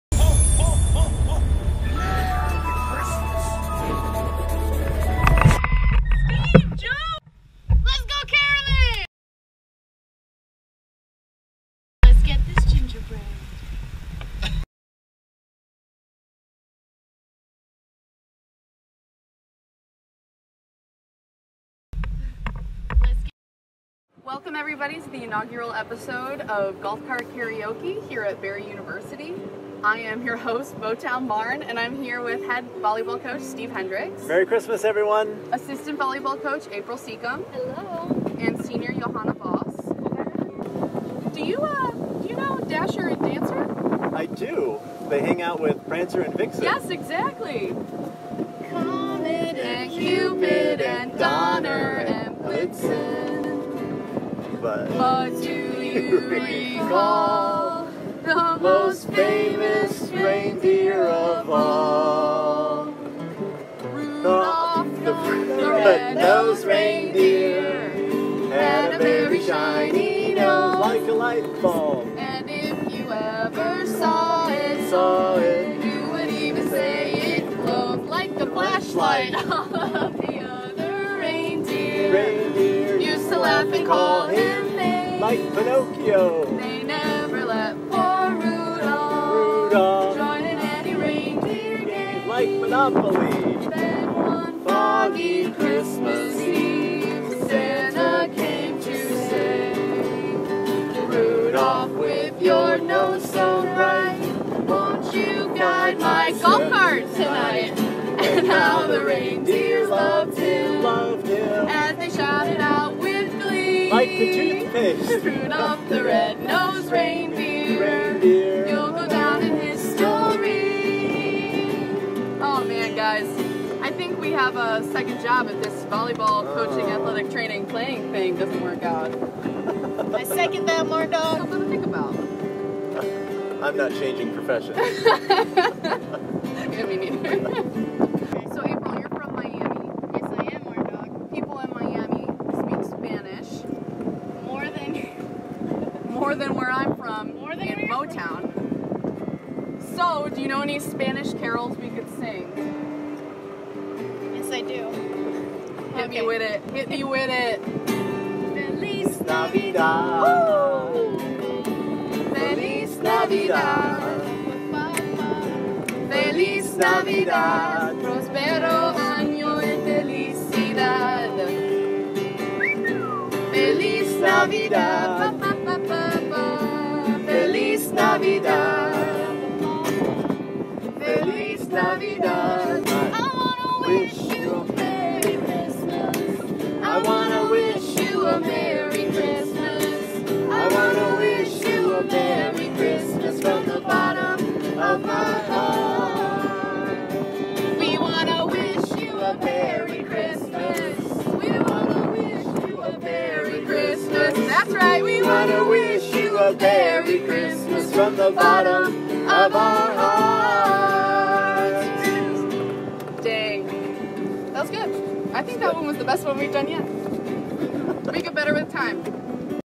BarryU Golf Cart Karaoke | Barry Christmas
The Buccaneer volleyball team kicks off the inaugural episode of our golf cart karaoke series with Christmas Cheer!